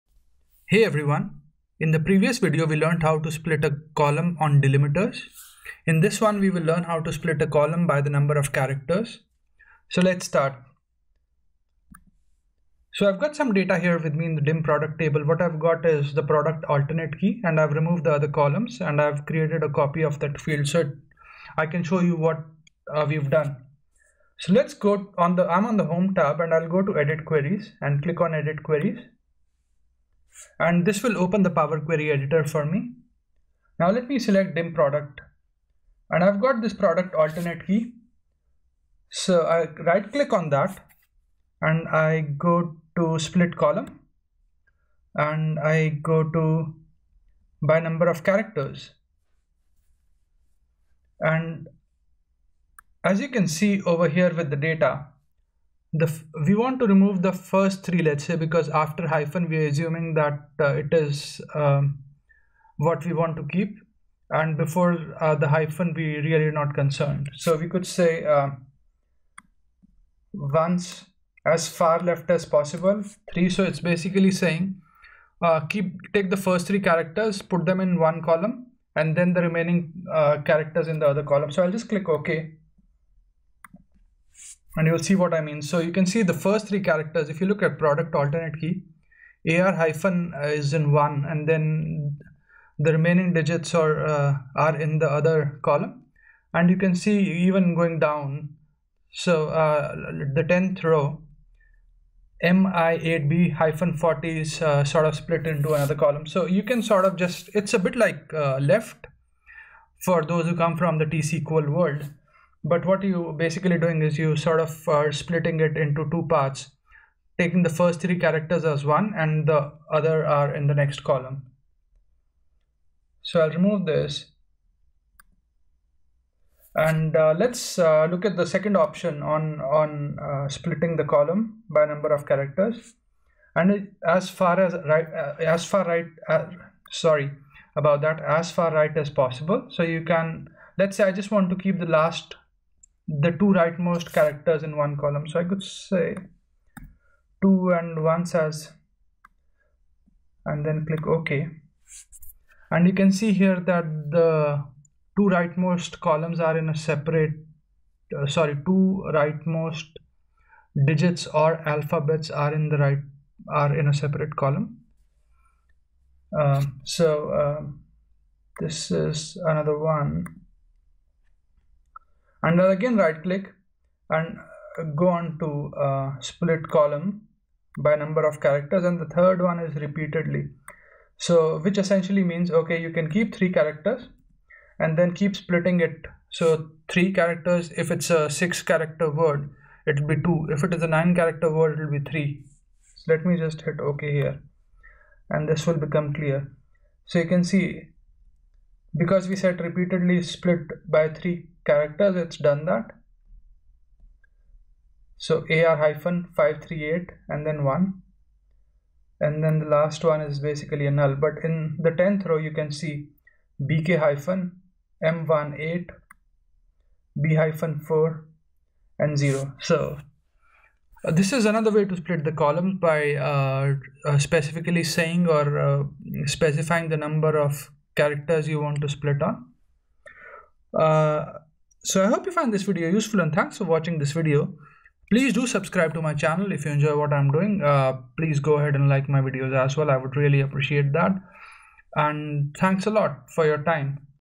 Split Column by Number Of Characters Power BI
Published on April 17, 2019:

In this video, we will learn to split columns by specifying the number of characters to split the column on.

In the previous video, we learnt to split columns using delimiters.

You can download power bi desktop from here.